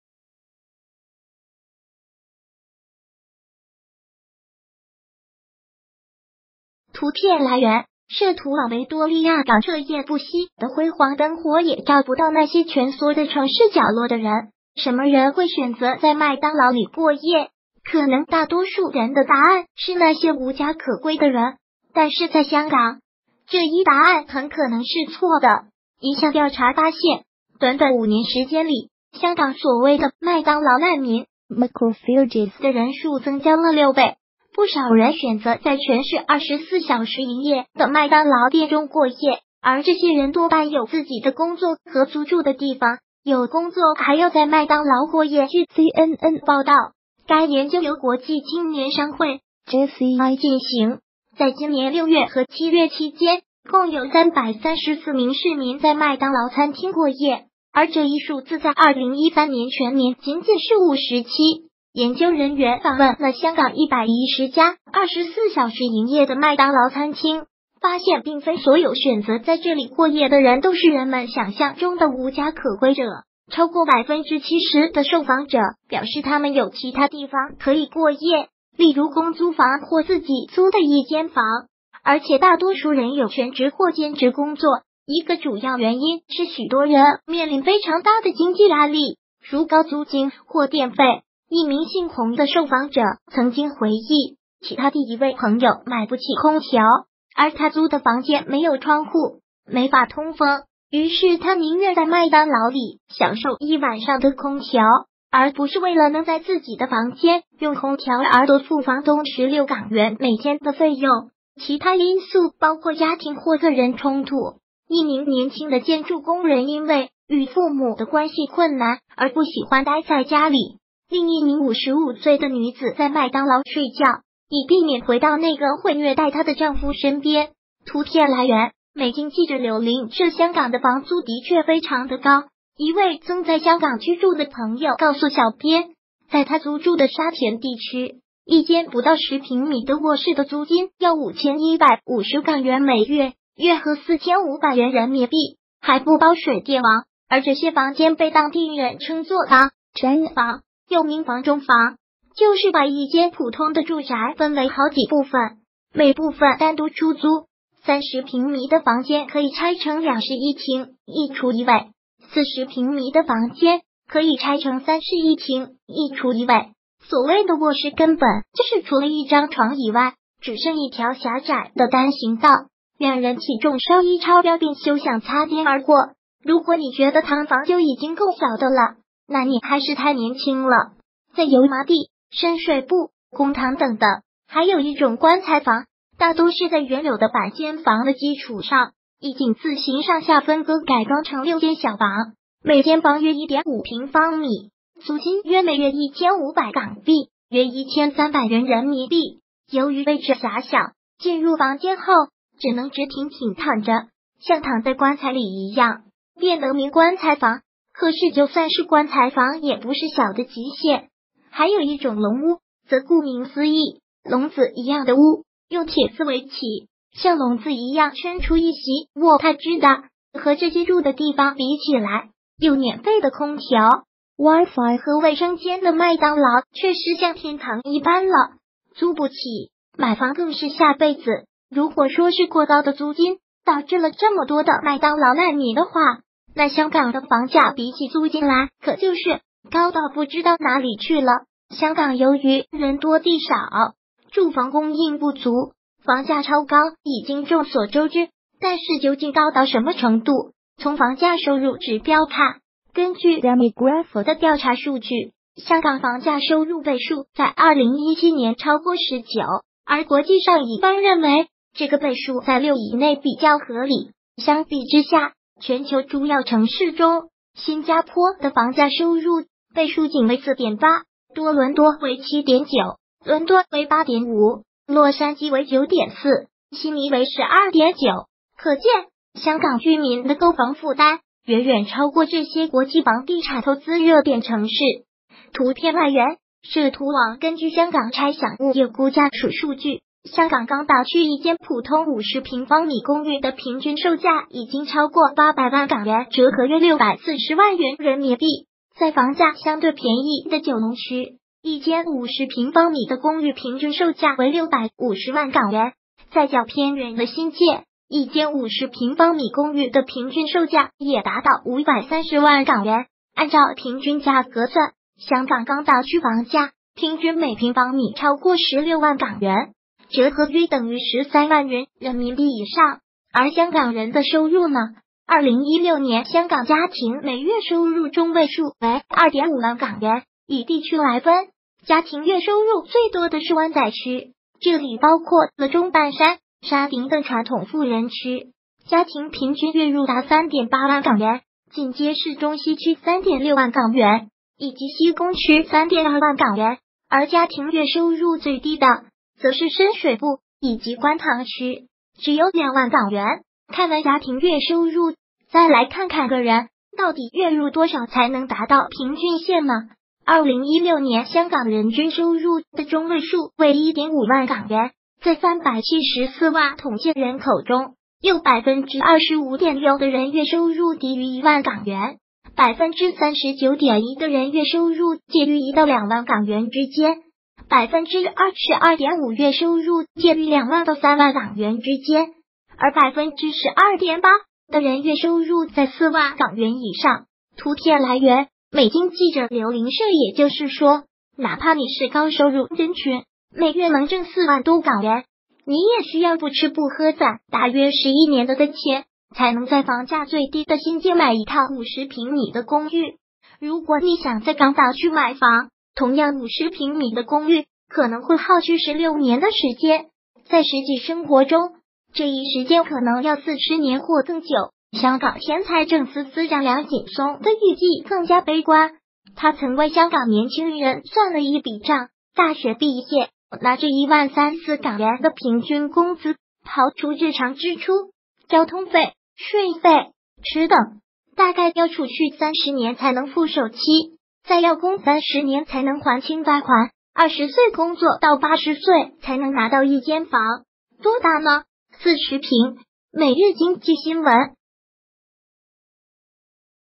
有工作有房子，这里的年轻人为什么彻夜守在麦当劳？
图片来源：摄图网维多利亚港彻夜不息的辉煌灯火也照不到那些蜷缩在城市角落的人。什么人会选择在麦当劳里过夜？可能大多数人的答案是那些无家可归的人，但是在香港，这一答案很可能是错的。一项调查发现，短短五年时间里，香港所谓的“麦当劳难民”（McRefugees）的人数增加了六倍——不少人选择在全市24小时营业的麦当劳店中过夜。而这些人多半有自己的工作和租住的地方。有工作还要在麦当劳过夜据CNN报道，该研究由国际青年商会（JCI）进行，在今年6月和7月期间共有334名市民在麦当劳餐厅过夜，而这一数字在2013年全年仅仅是57。研究人员访问了香港110家24小时营业的麦当劳餐厅，发现并非所有选择在这里过夜的人都是人们想象中的无家可归者。超过70％的受访者表示他们有其他地方可以过夜，例如公租房或自己租的一间房，而且大多数人有全职或兼职工作。一个主要原因是许多人面临非常大的经济压力，如高租金或电费。一名姓洪的受访者曾经回忆起他的一位朋友买不起空调，而他租的房间没有窗户，没法通风。于是他宁愿在麦当劳里享受一晚上的空调，而不是为了能在自己的房间用空调而多付房东16港元每天的费用。其他因素包括家庭或个人冲突